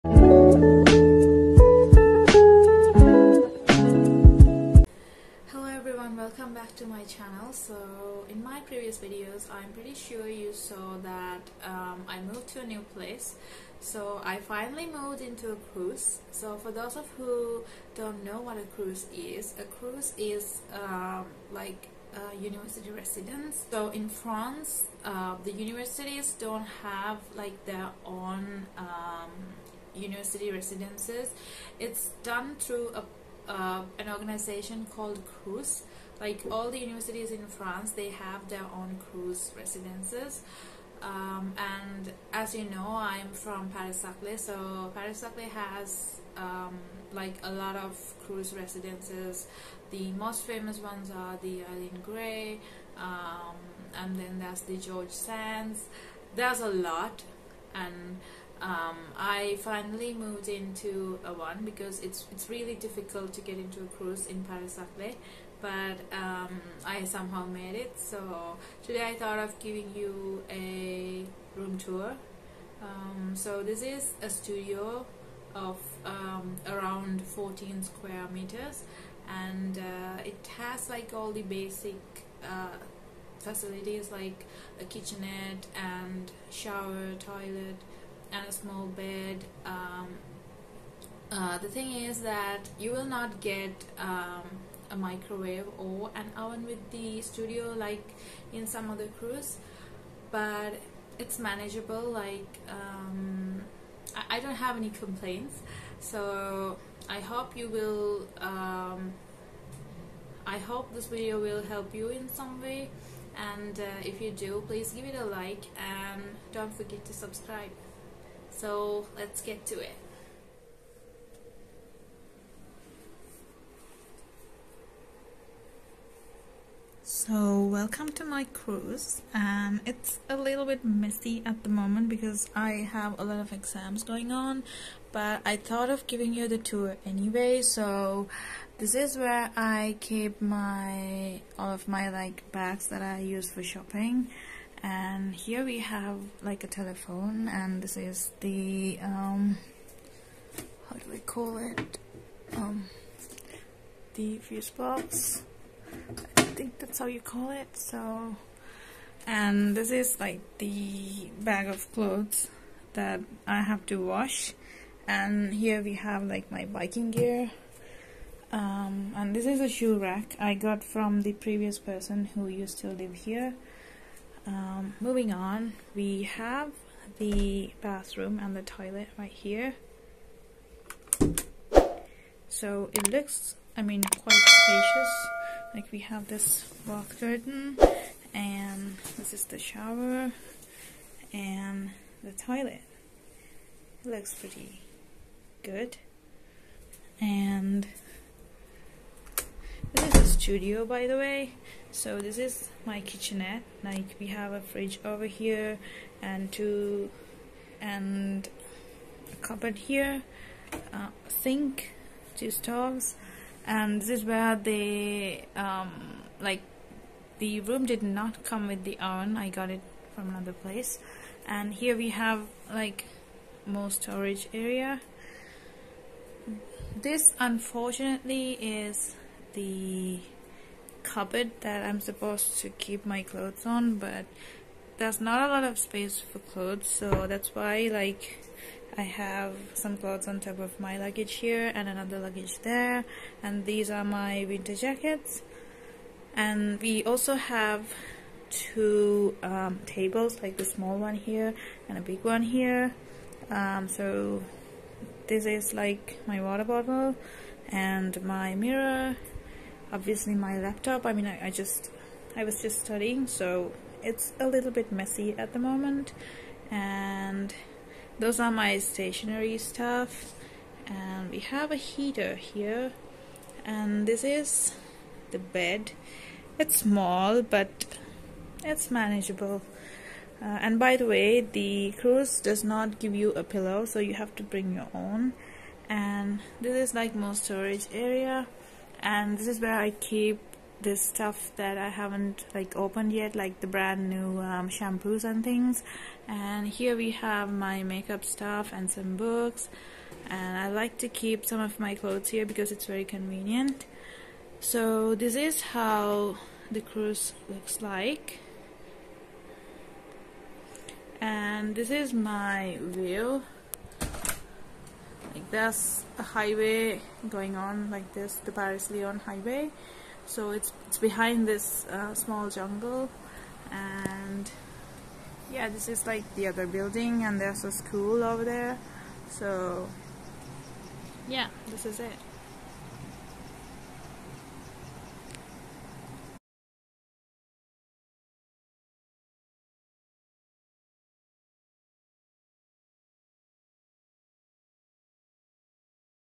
Moving into my new studio | Crous Paris Saclay| Crous Residence room tour| Sri Lankan student vlog
So in todays video I decided to do a vlog about my new house, a studio apartment in a Crous residence at Paris Saclay University.

Copyright @ ruvisthoughts.
Any reproduction, illegal distribution or using this video or a part of this video without the authorization of the creators is prohibited.